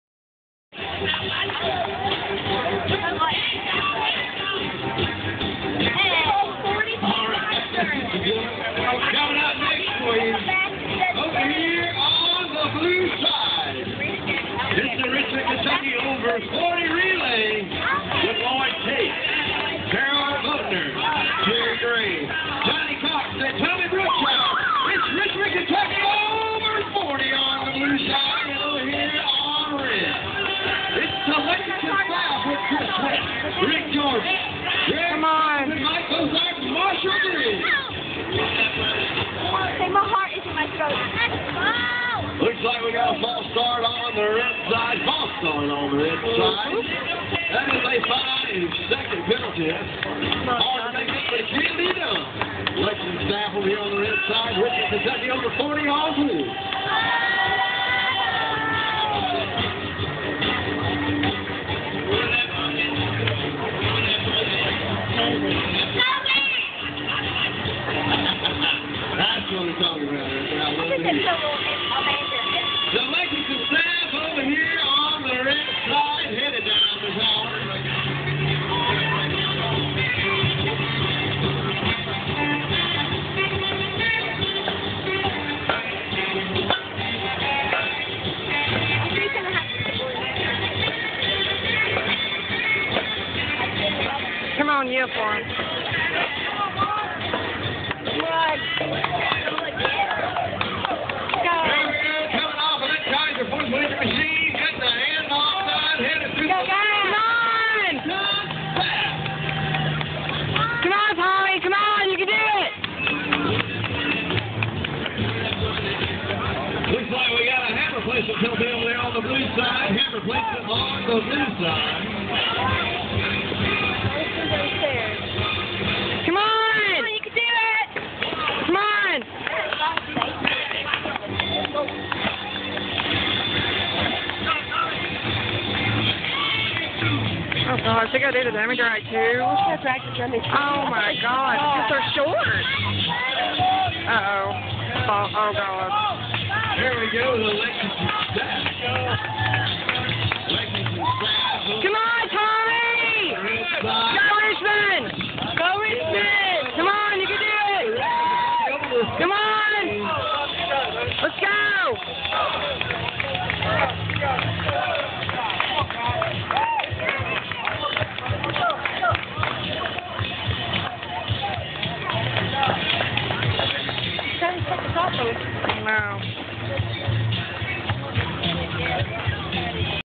Scott Firefighter Combat Challenge Lexington 2011
Tommy Brookshire and Richmond Ky Firefighters round 1